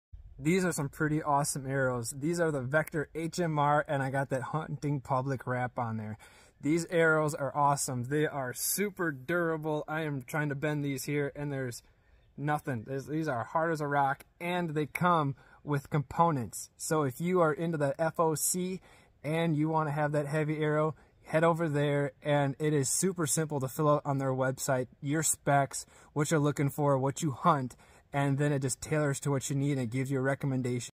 Best Hunting Arrows: 2022
These seem to be the top performing and best hunting arrows on the market for 2022. They also come in at a very affordable price, which makes them very budget friendly arrows.

I have shot these arrows a handful of times at the range. They are hard hitting, have great penetration, and are very durable.

These arrows come with the FOC components already installed. So all you have to do is remove the screw that it comes with and then screw in your field point or broadhead. Field points are not included, in the video, the field point on my arrow is my own.

These will be the arrows I will be using this fall for this upcoming 2022 deer archery season. I am super excited to use these arrows and I highly recommend checking these arrows out.

Vector Custom Shop Arrows - (HMR) with The Hunting Public Wrap -

Drop your comments! I want to know what arrows you will be shooting!